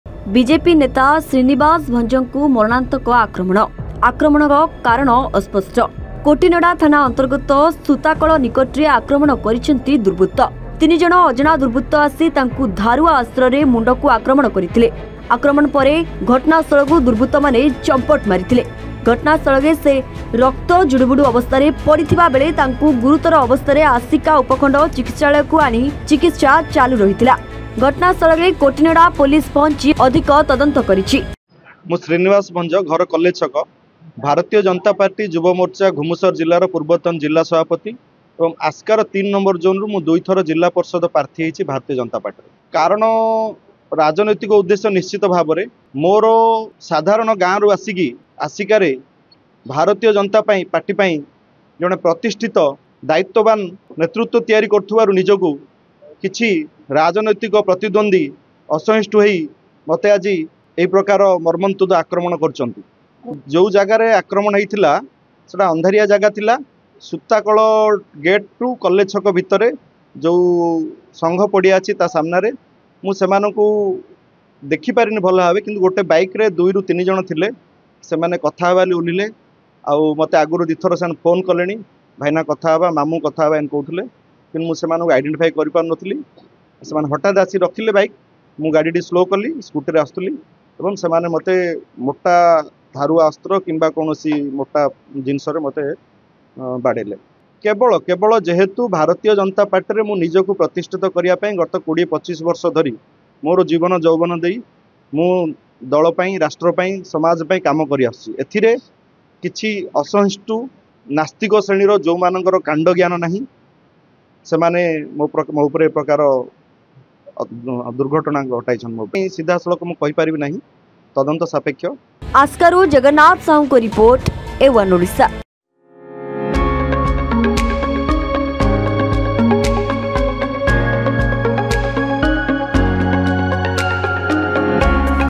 ୩ ଅଜଣା ଦୁର୍ବୁତ୍ତ ବିଜେପି ନେତାଙ୍କ ମୁଣ୍ଡକୁ ଆକ୍ରମଣ କରି ଘଟଣା ସ୍ଥଳରୁ ଫେରାର | GANJAM NEWS
୩ ଅଜଣା ଦୁର୍ବୁତ୍ତ ବିଜେପି ନେତାଙ୍କ ମୁଣ୍ଡକୁ ଆକ୍ରମଣ କରି ଘଟଣା ସ୍ଥଳରୁ ଫେରାର | @a1odisha